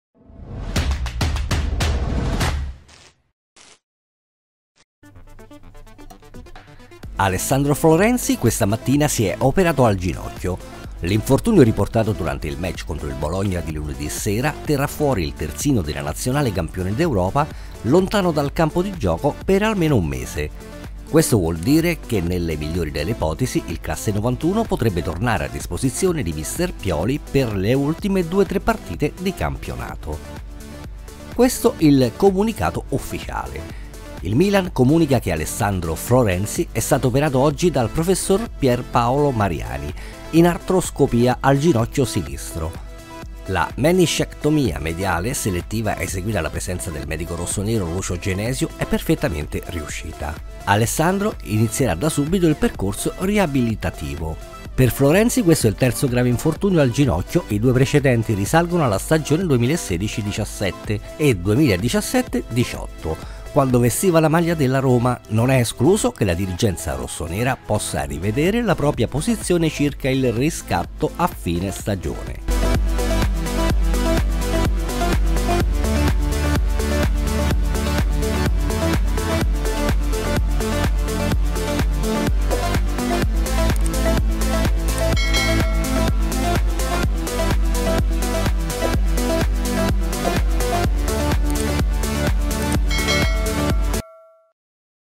ALESSANDRO FLORENZI: Operato questa mattina al ginocchio dal Prof. Mariani
AC Milan comunica che Alessandro Florenzi è stato operato oggi dal Professor Pier Paolo Mariani in artroscopia al ginocchio sinistro. La meniscectomia mediale selettiva, eseguita alla presenza del medico rossonero Lucio Genesio, è perfettamente riuscita. Alessandro inizierà da subito il percorso riabilitativo.